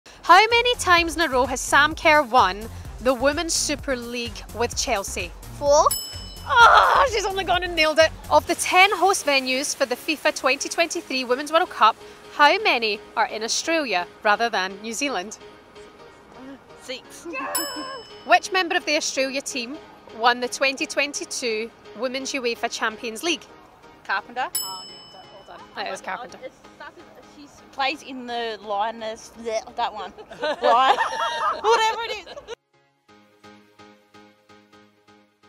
Can Australia fans pass our Matilda's quiz?
We questioned Australia fans about Australia's CommBank Matildas outside the stadium ahead of Australia's FIFAWWC match against Nigeria.